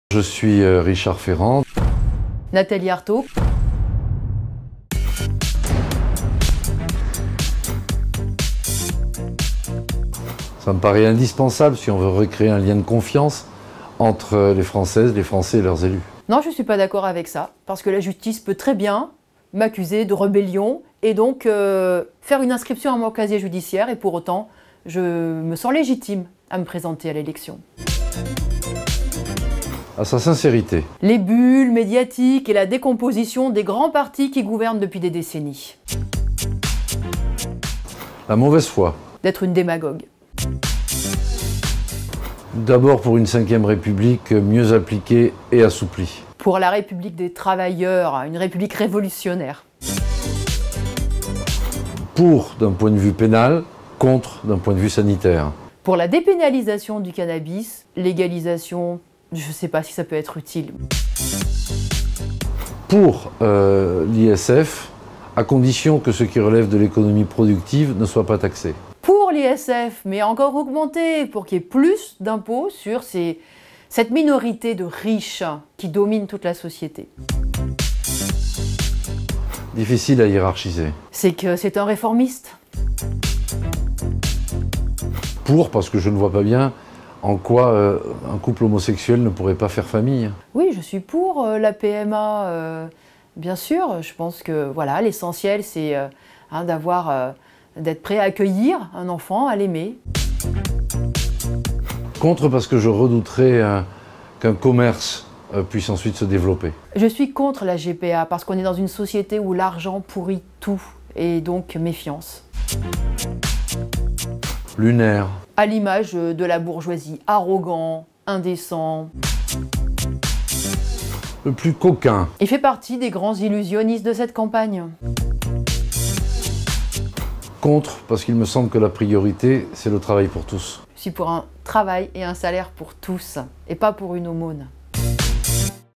Duel : Richard Ferrand VS Nathalie Arthaud
On a posé les mêmes questions à Richard Ferrand de En Marche et Nathalie Arthaud, candidate Lutte Ouvrière à la présidentielle, et bizarrement... Ils sont parfois d'accord. Duel.